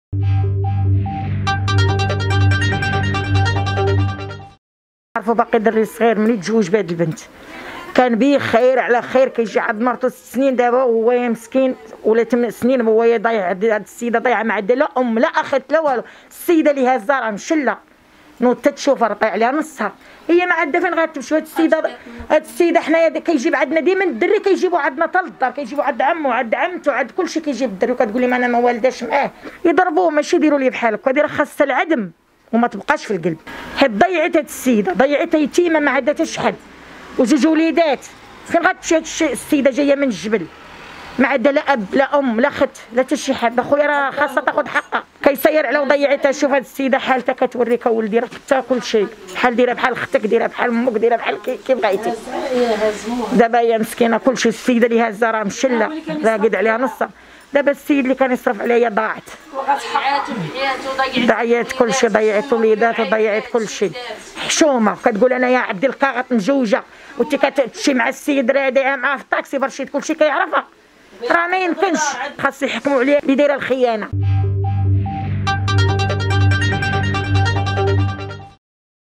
جارة الشاب لي مات برصاص الدرك بالدروة تفجرها:"الشابة صاحبتو والرضيع لي خطفو ولدو"
اضطر دركي إلى استعمال سلاحه الوظيفي لتوقيف شخص قام باختطاف رضيع وعرض أمن وسلامة عناصر الدرك الملكي لتهديد خطير بواسطة سلاح أبيض